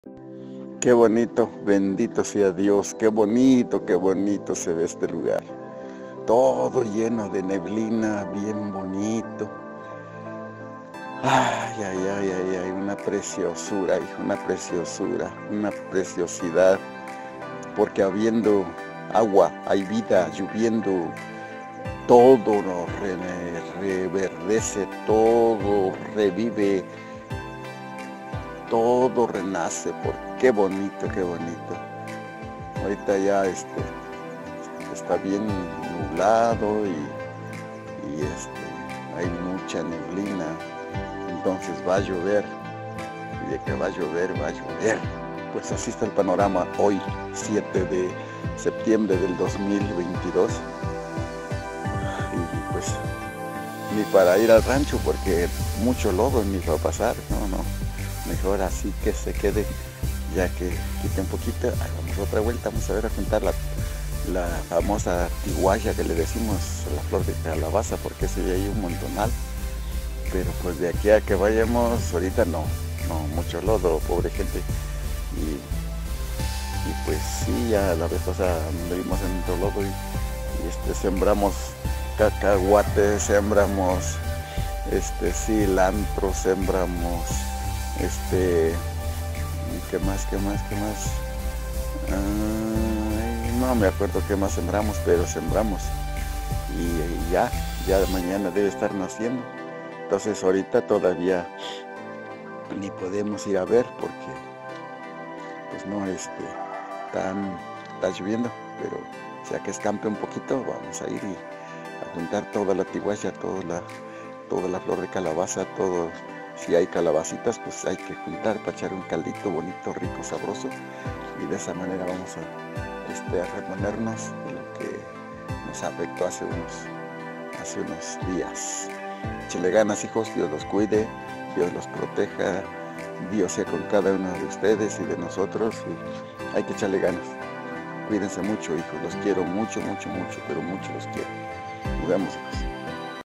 Dia nublado
La temporada de lluvias embellece los campos, los montes y sobre todo el humor de los seres vivos y mas importantes del ser humanos